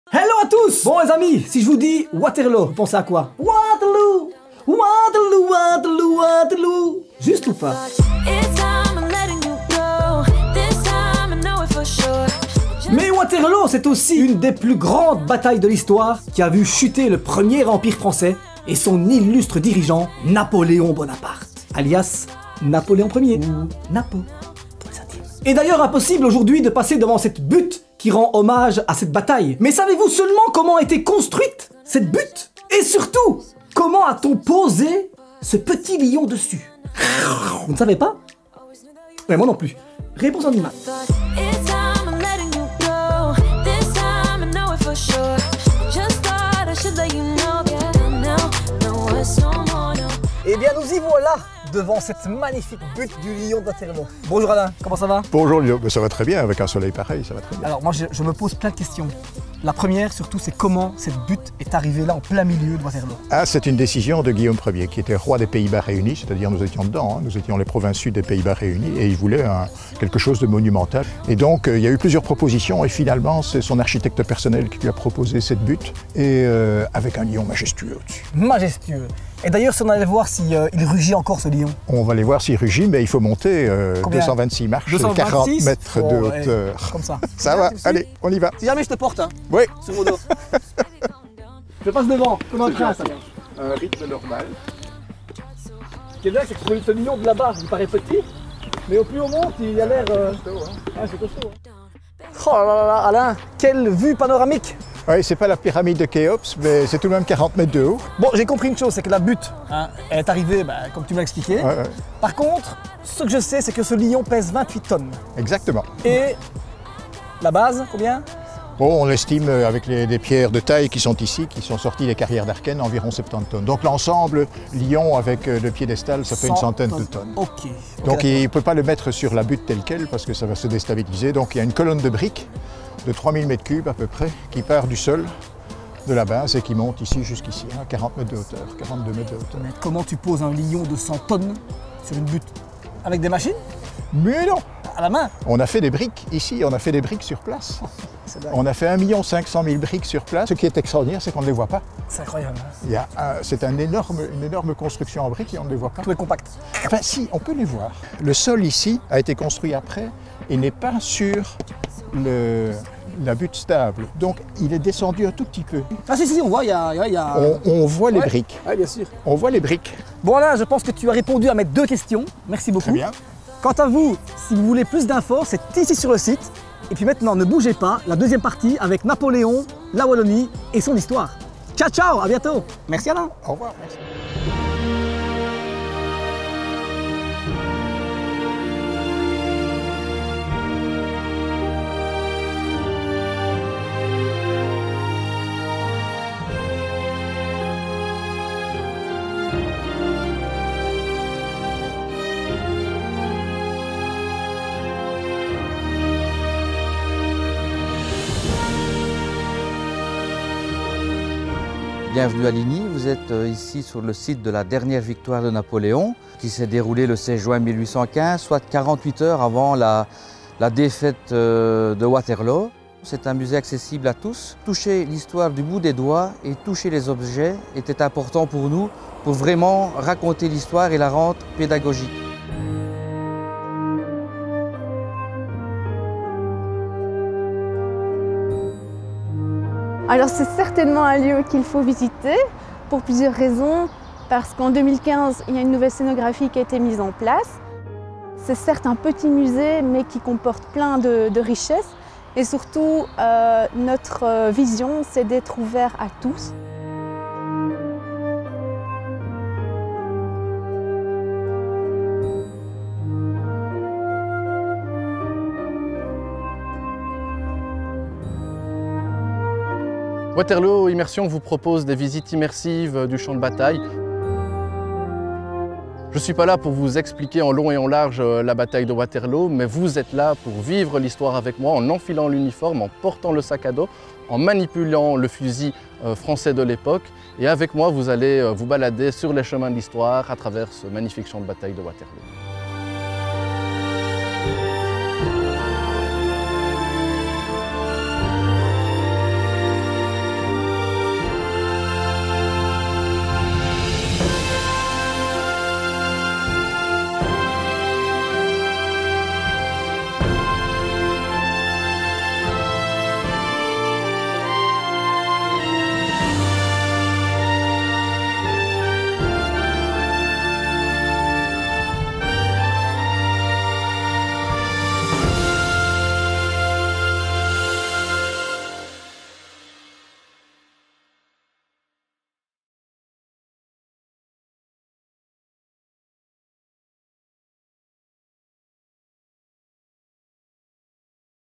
La Bataille de Waterloo et la Butte du Lion | Les Découvertes de Ludo
Ludo vous raconte l’histoire de la Butte du Lion, le monument emblématique de la Bataille de Waterloo. Ensuite, il vous emmène sur la Route Napoléon, où d’autres lieux témoignent de l’épopée napoléonienne en Wallonie. Alors, êtes-vous prêts pour voyager dans le temps ?